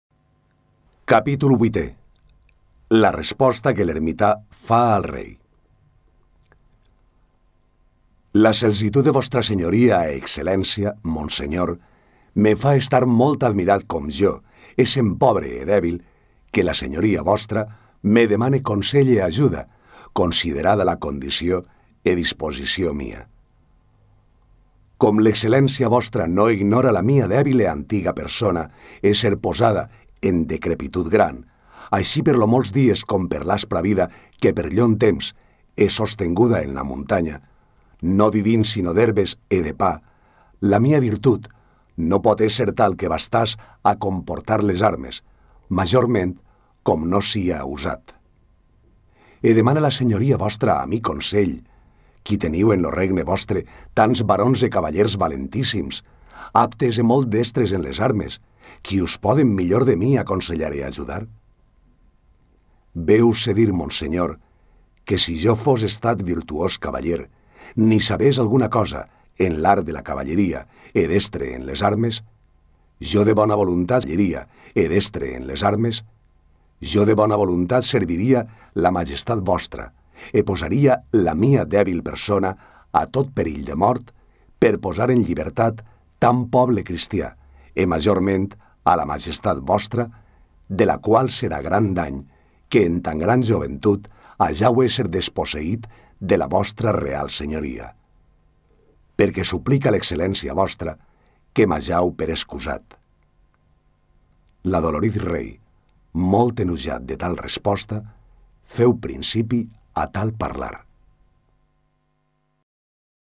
Tirant lo Blanc: Capítol VIII
Àudio. Tirant lo Blanc: Capítol VIII. Transcripció de l'exemplar conegut com N1 o NY1 de l'edició de València 1490, feta per Nicolau Spindeler, han col·laborat en aquest treball Eduard Baile, Jordi Segura, Héctor Cámara; supervisió Llúcia Martín. Biblioteca Virtual Miguel de Cervantes. Biblioteca Virtual Joan Lluís Vives.